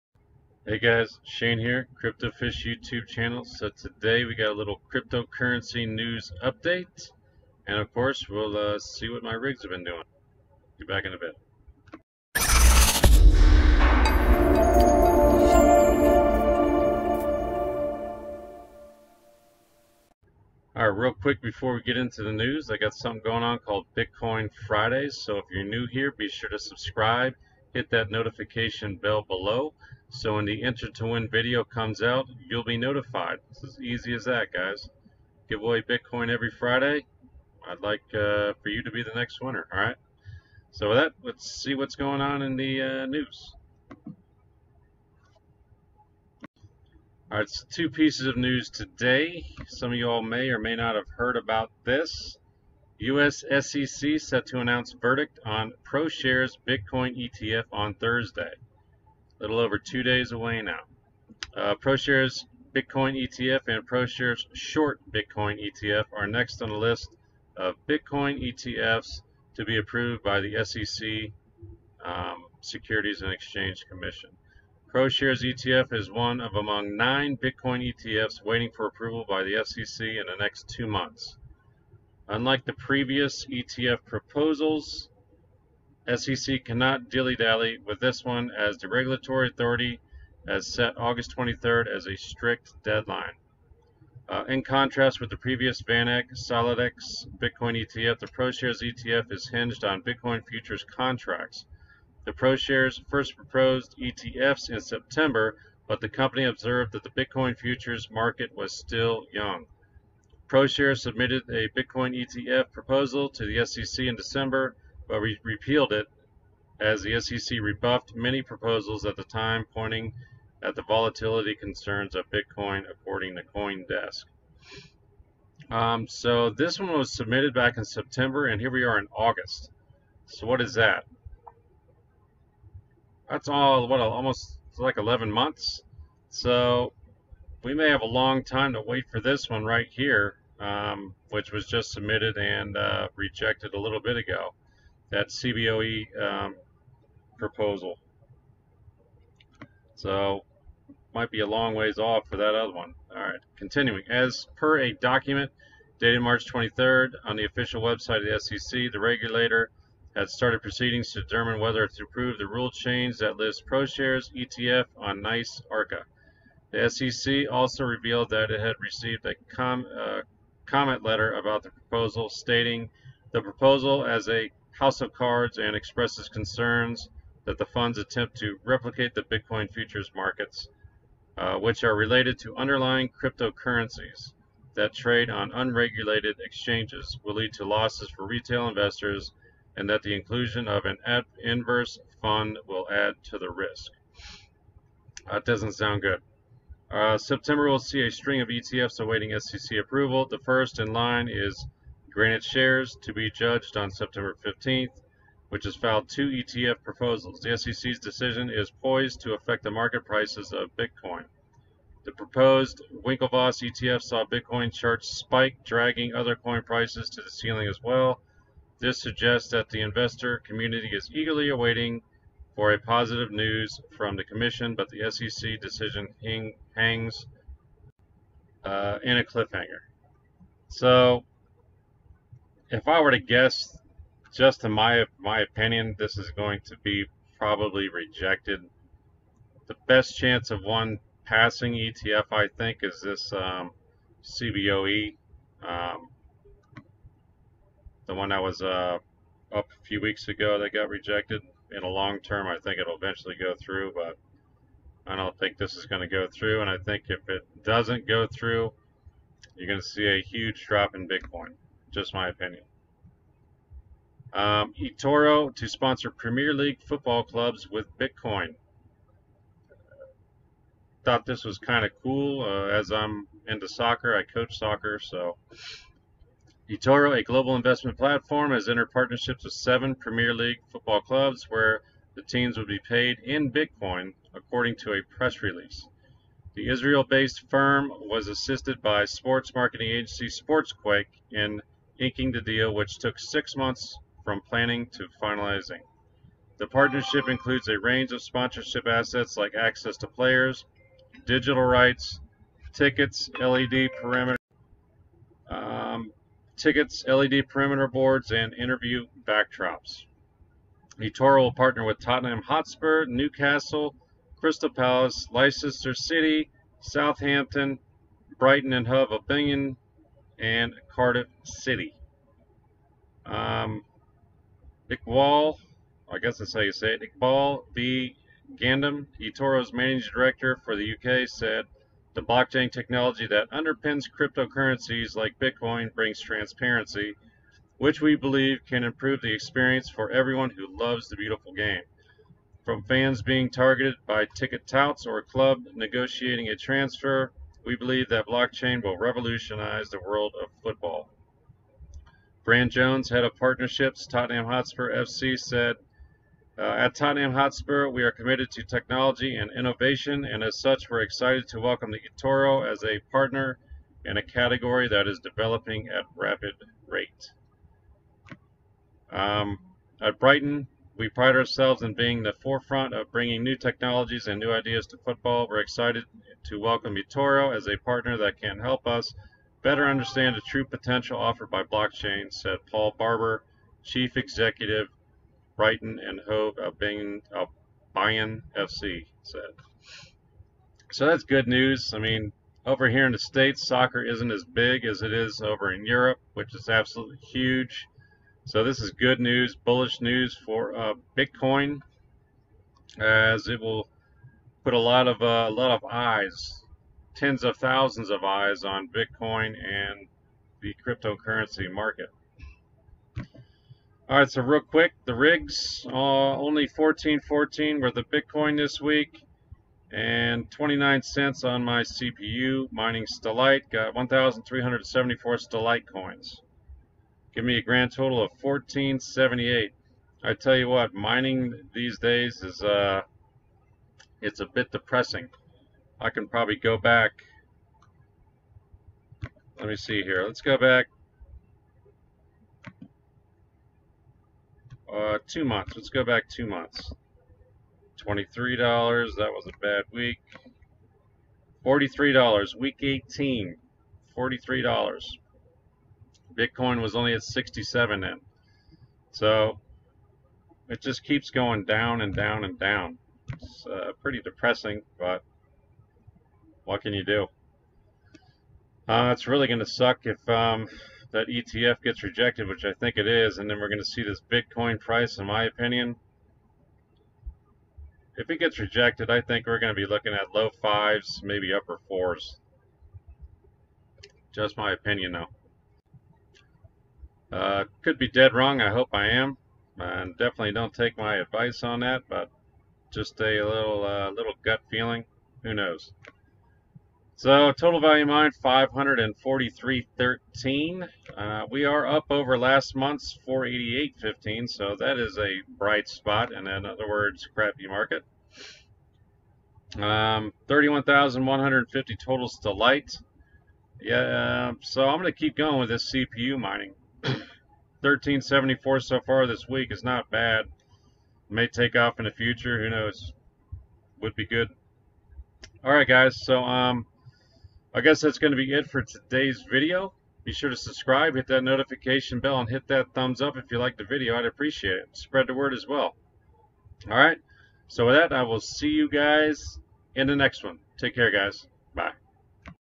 Cryptocurrency News Update - SEC ETF and Premier League Sponsor
Today's cryptocurrency news update involves the Proshares Bitcoin ETF ruling this Thursday August 23rd.  Will it be approved?  My thoughts are no but only time will tell.  Also, some bullish bitcoin news comes to us via a Premier League sponsorship by eToro.  eToro plans to pay, in Bitcoin, 6 different premier league clubs for it's ability to advertise at  there stadiums.  Great news for the market but that could be overshadowed immensely with the SEC ruling this week.